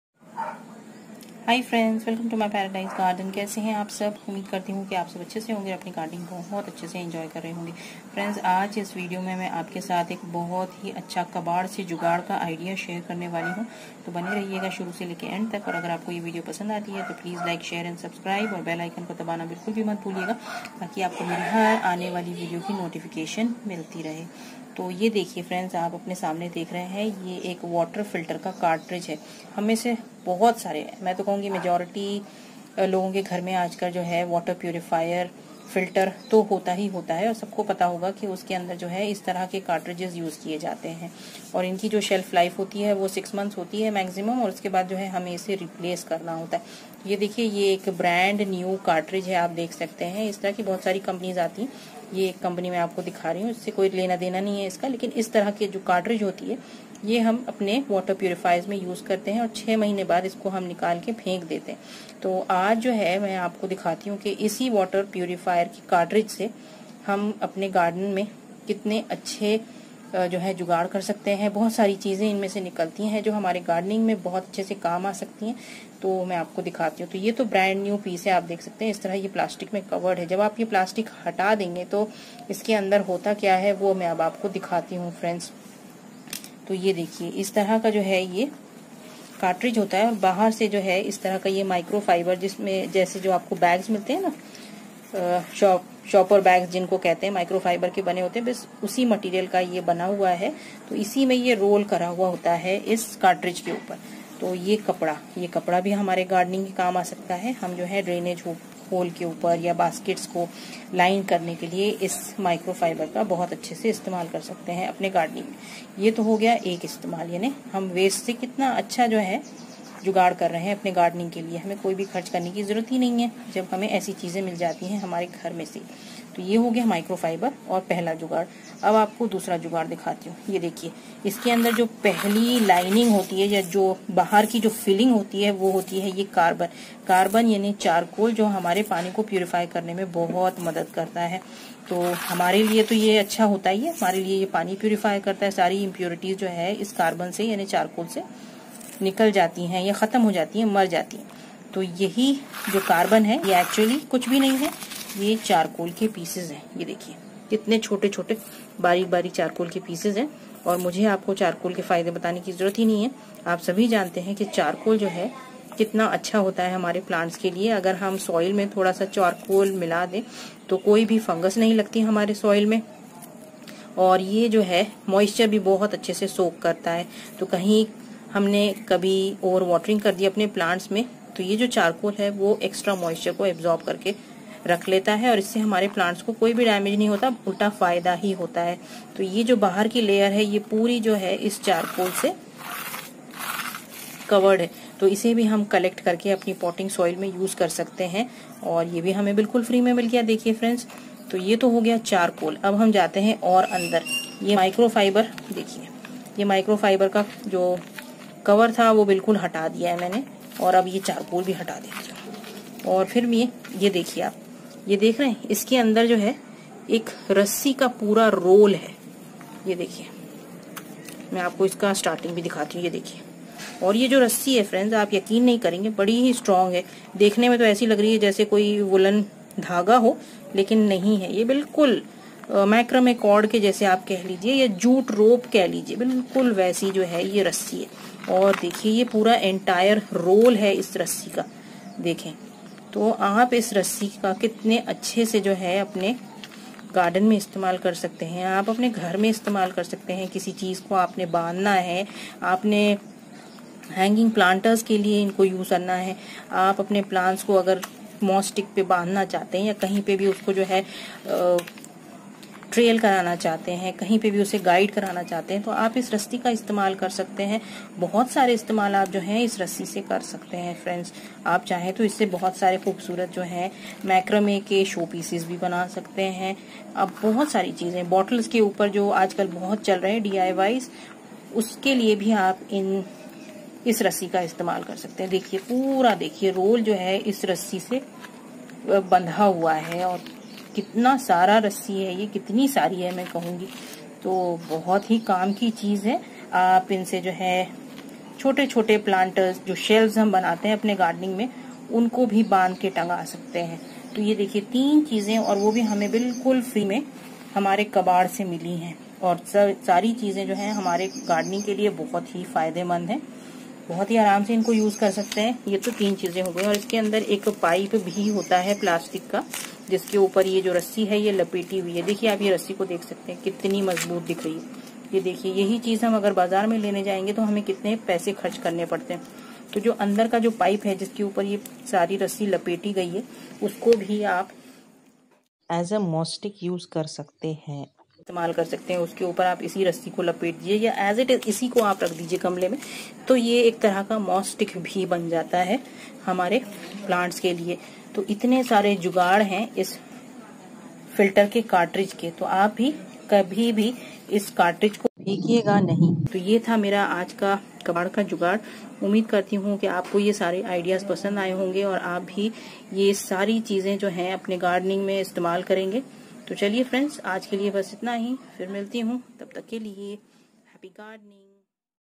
Zabardast kabaad se jugaad Aapke garden ke liye/ Reuse Recycle Reduce waste Water Filter Catridge
Happy Gardening friends 🌱❤️🌱